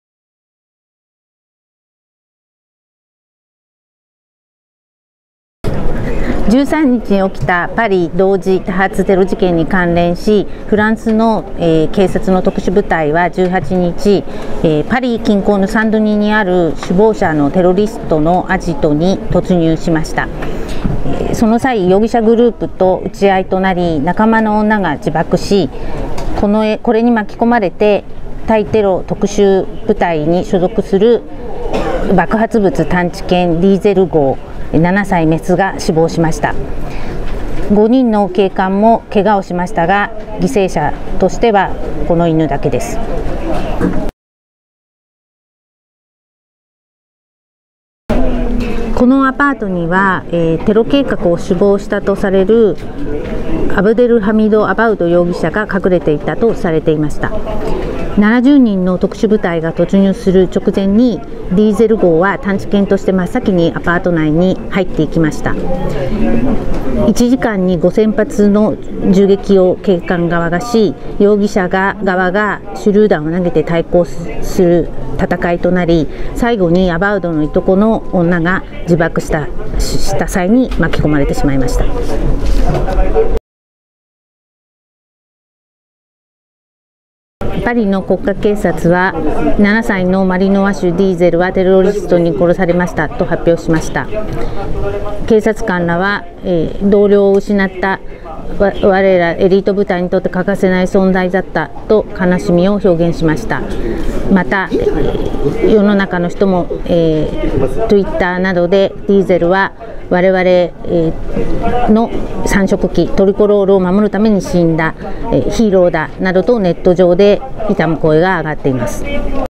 注目ニュース90秒 パリ同時多発テロ 爆発物探知犬が犠牲に
パリ同時多発テロを巡る１８日の治安部隊の急襲で、警察側で唯一の犠牲になったのが特殊部隊「ＲＡＩＤ」に所属する爆発物探知犬「ディーゼル」号（７歳、メス）だった。「作戦には必要不可欠な犬だった」とフランス警察がその死を悼んだディーゼルについて、外信部の國枝すみれデスクに聞いた。